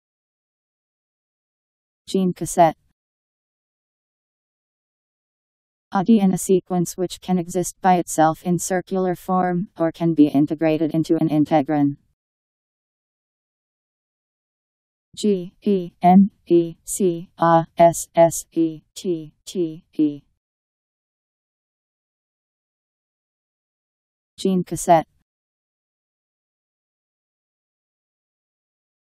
Gene cassette Meaning
Video shows what gene cassette means. A DNA sequence which can exist by itself in circular form, or can be integrated into an integron..  Gene cassette Meaning. How to pronounce, definition audio dictionary. How to say gene cassette. Powered by MaryTTS, Wiktionary